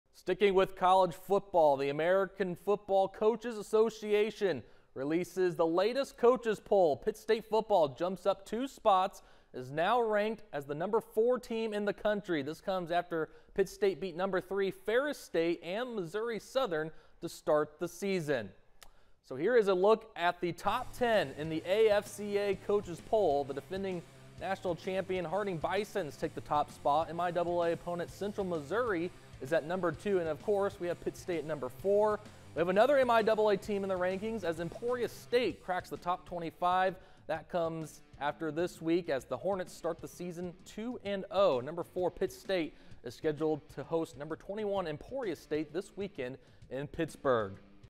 Pitt State Football cracks the Top 5 in new rankings
Gorillas move up to #4 in the country